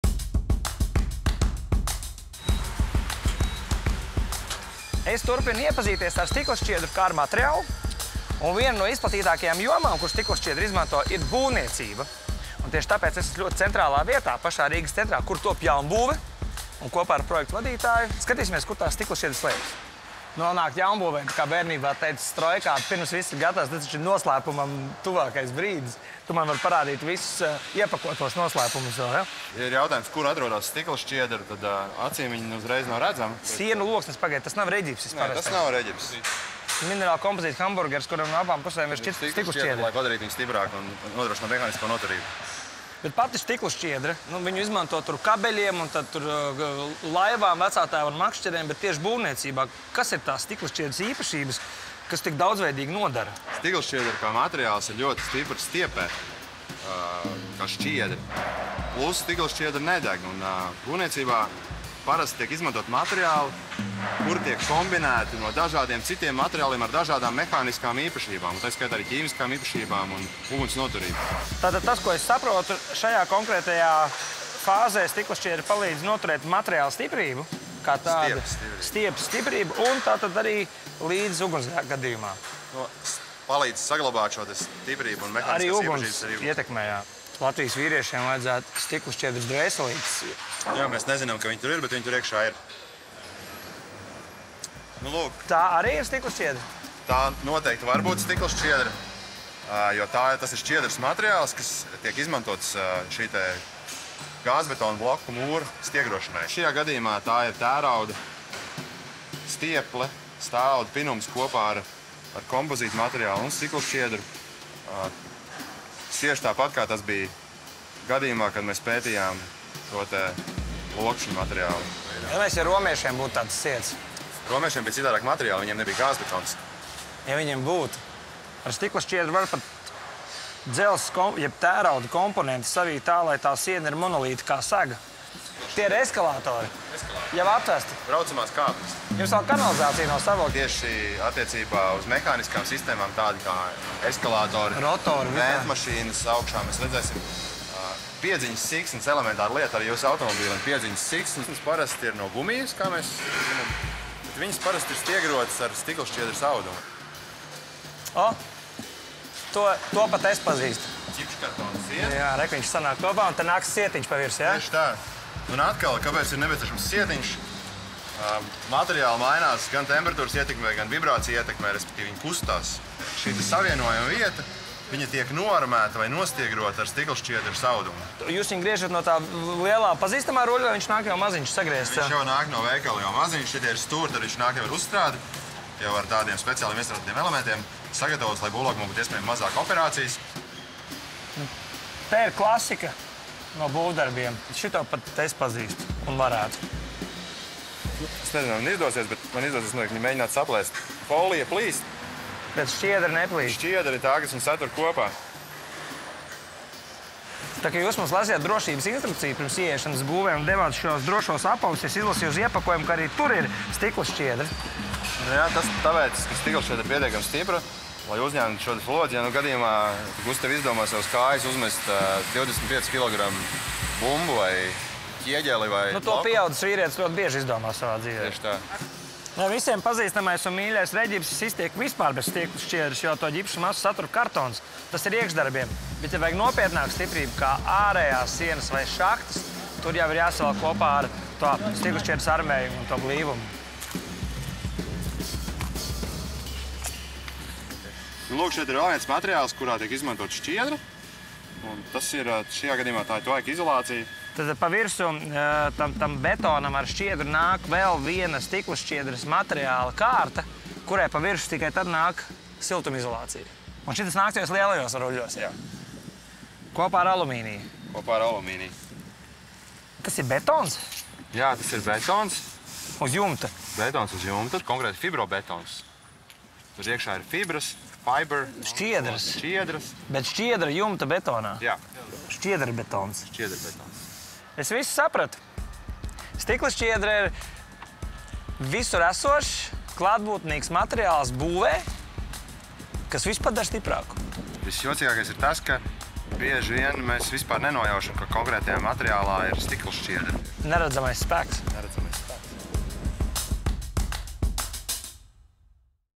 VALMIERA GLASS: The invisible force
Glass fibre is versatile material with rare and outstanding qualities like strength, flexibility, non-combustibility, water and chemical resistance, as well as resistance to chemicals and biological effects.

Because of all these properties glass fibre is commonly used in construction.   Although widely used, it is rarely obvious. Follow Gustavs Terzens in a new building in the very heart of Riga and find out where the invisible force of glass fibre lies!